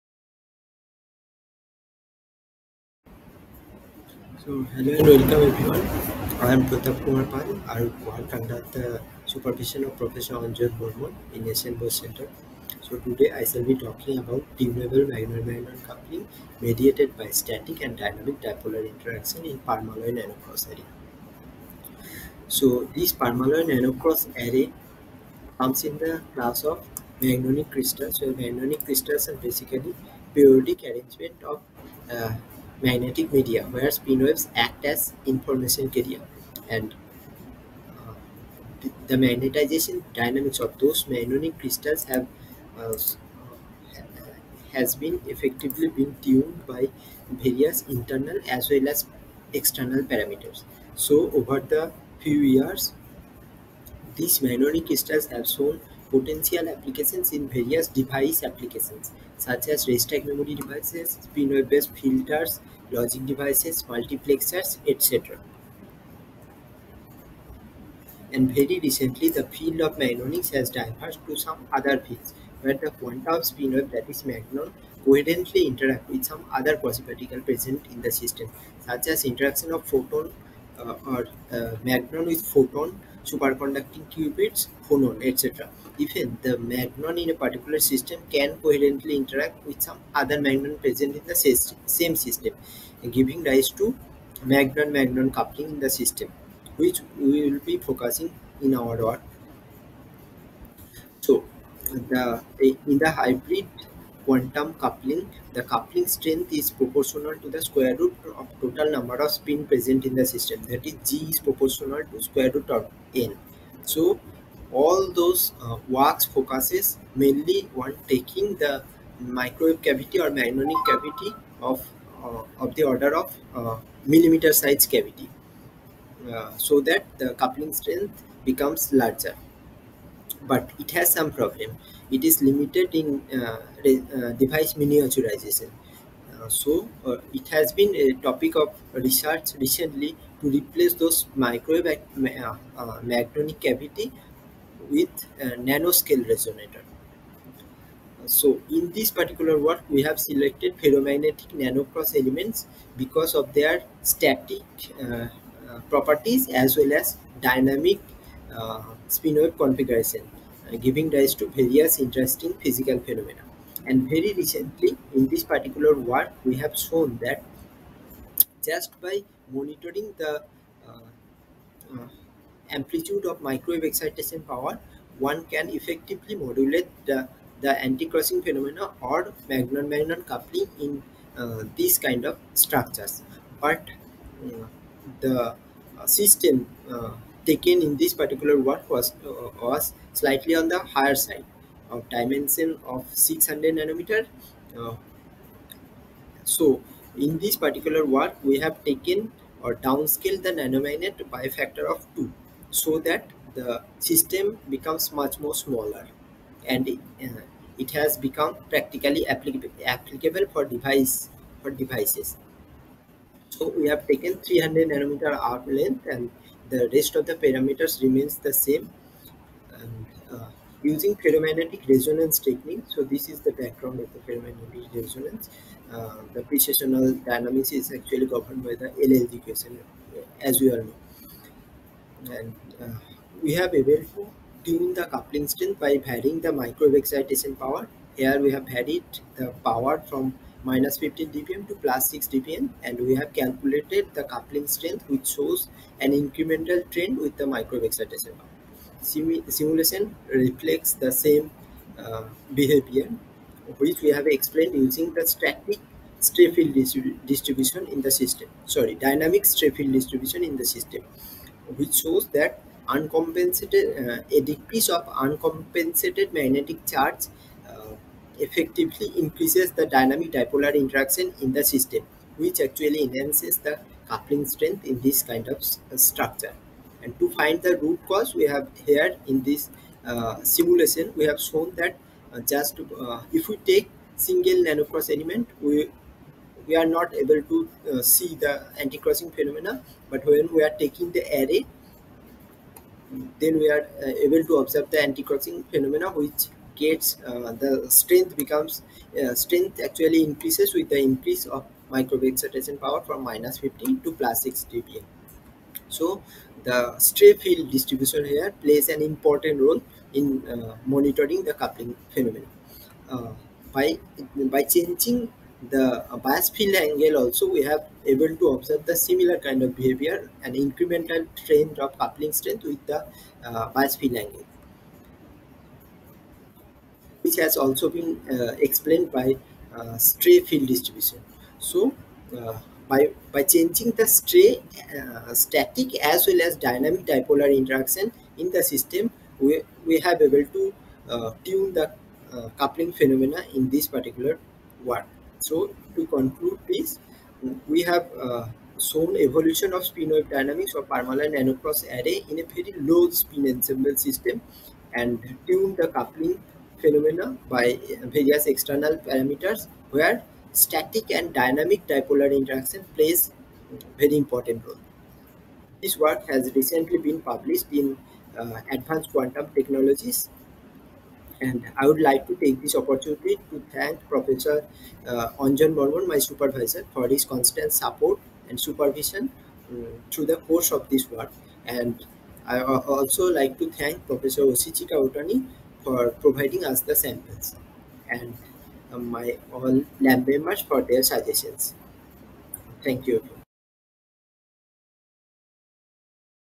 IEEE Mag Summer School 2023-Pal Pratap Kumar, S.N. Bose Nat. Cen for Basic Sciences, Kolkata, India
- Student presentation given at the 2023 IEEE Magnetics Society Summer School (Carovigno-Bari, Italy, June 11-16, 2023).